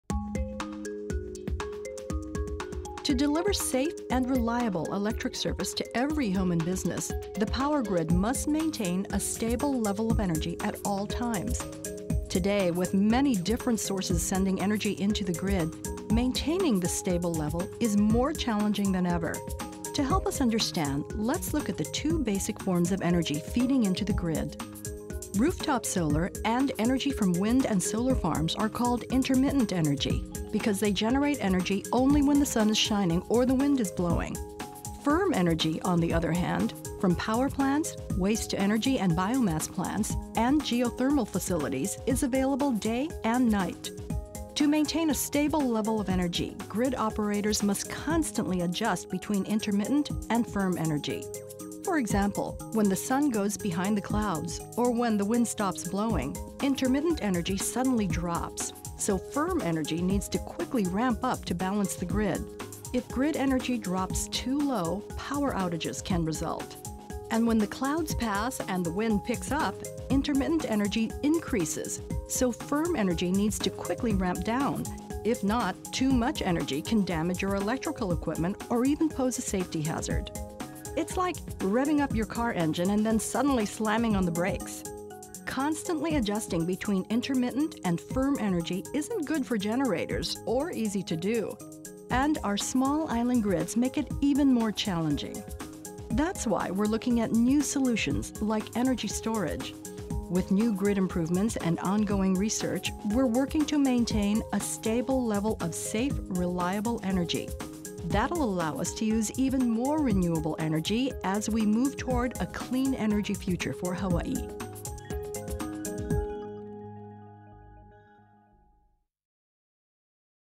Maintaining Safe and Reliable Energy for Customers
To deliver safe and reliable electric service to every home and business, the power grid must maintain a stable level of energy at all times.  Today, with many different sources sending energy into the grid, maintaining the stable level is more challenging than ever for the Hawaiian Electric Companies.  Grid operators must constantly adjust between intermittent (rooftop solar and wind) and firm energy to balance the grid.